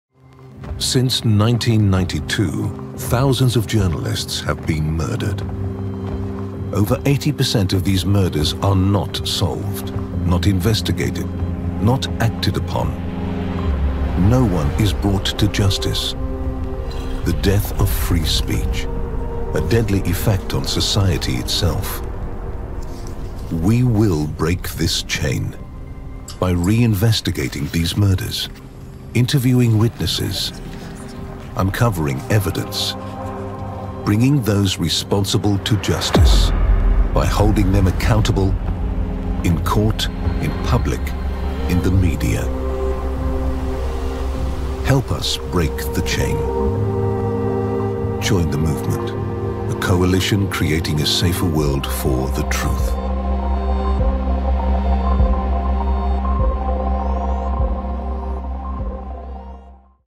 A Safer World For the Truth Explainer video
Every single week a journalist is killed. Killing the truth is the world’s safest crime – in more than eight out of ten cases the killers go free. Impunity sends the message that murdering journalists goes unpunished. It must end. That is why Free Press Unlimited launched the A Safer World For The Truth project, together with Committee to Protect Journalists (CPJ) and Reporters Without Borders (RSF). To investigate. To document. To pursue justice. And create A Safer World for the Truth. We do this by re-investigating murders and reporting our findings to the media, civil society and the authorities to advocate for justice.

This video explains what we do.